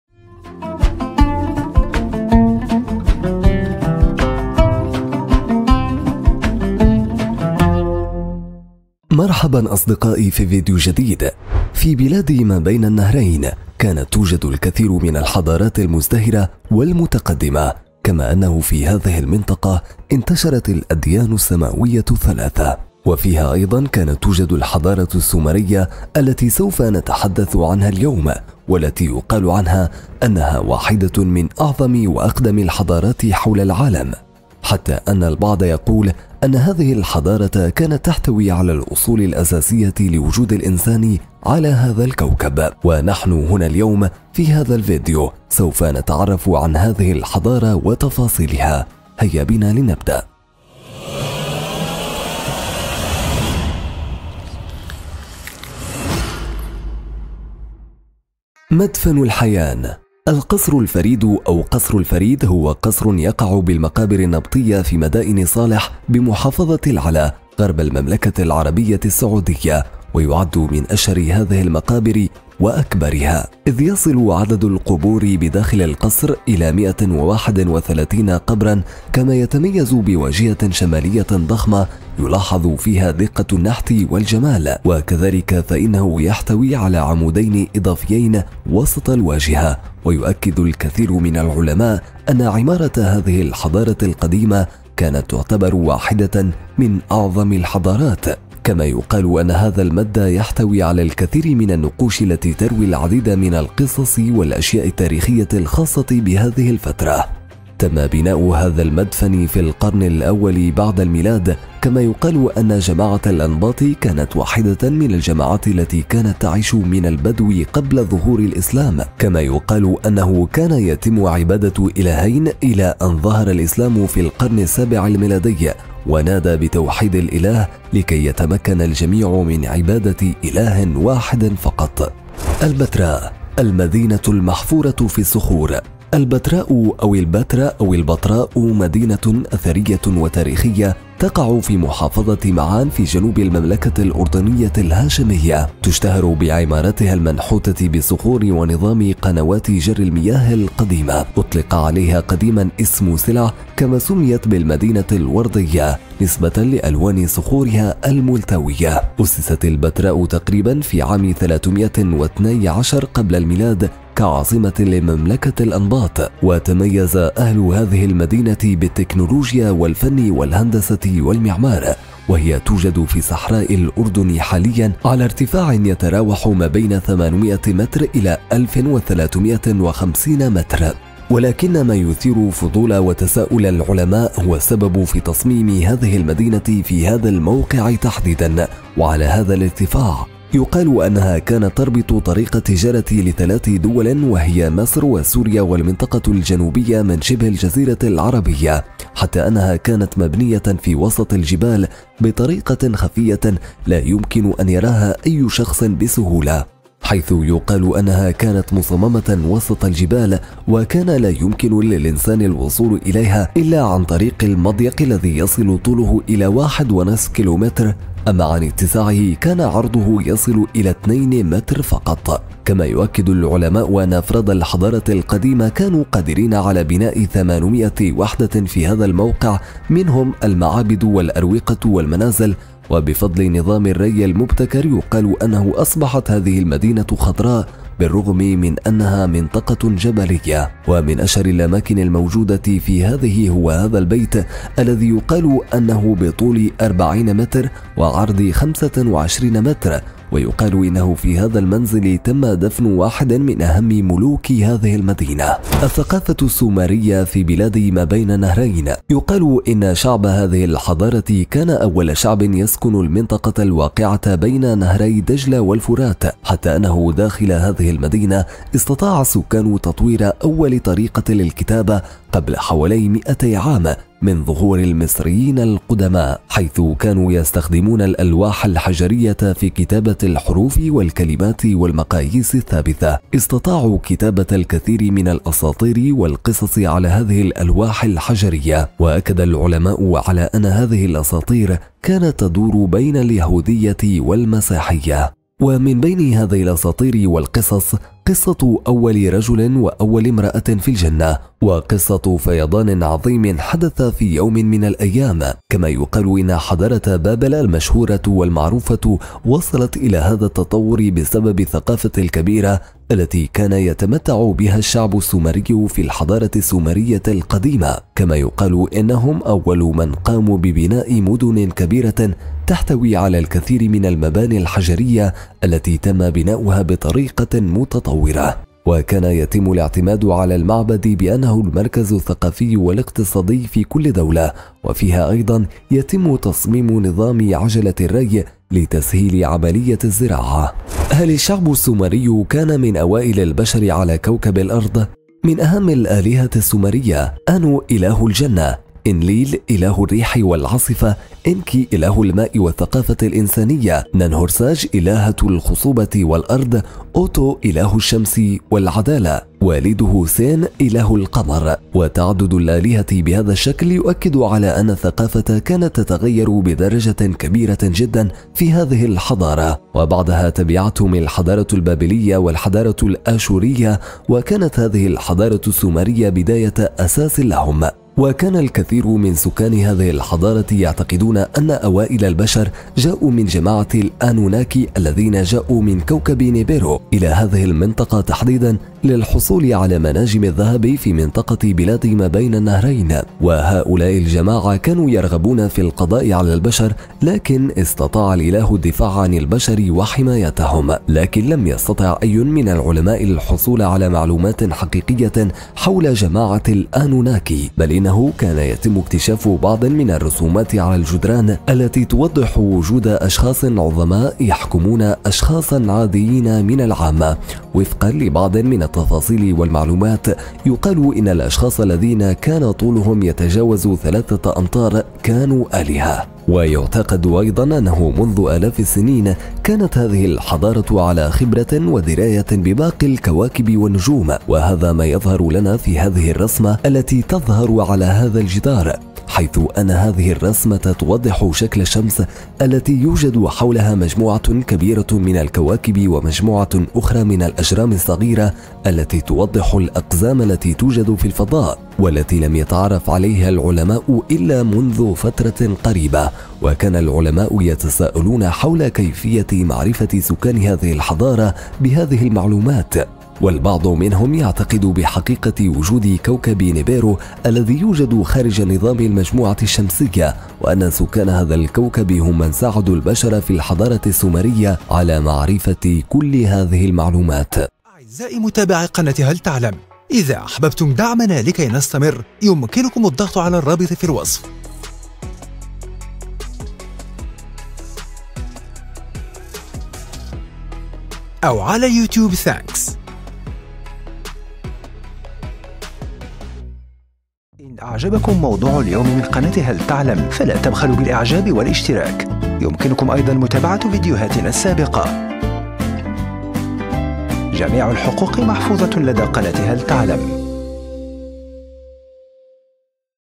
اكتشاف غامض في صحاري العراق قلب فهم العلماء لتاريخ الإنسان رأساً على عقب !!
مرحبا اصدقائي في فيديو جديد.. في بلاد ما بين النهرين كانت توجد الكثير من الحضارات المزدهرة والمتقدمة، كما انه في هذه المنطقة انتشرت الاديان السماوية الثلاثة. وفيها ايضا كانت توجد الحضارة السومرية التي سوف نتحدث عنها اليوم والتي يقال عنها انها واحده من أعظم وأقدم الحضارات حول العالم، حتى ان البعض يقول لان هذه الحضارة كانت تحتوي على الاصول الأساسية لوجود الانسان على هذا الكوكب. ونحن هنا اليوم في هذا الفيديو سوف نتعرف على هذه الحضارة وتفاصيلها. هيا بنا نبدا.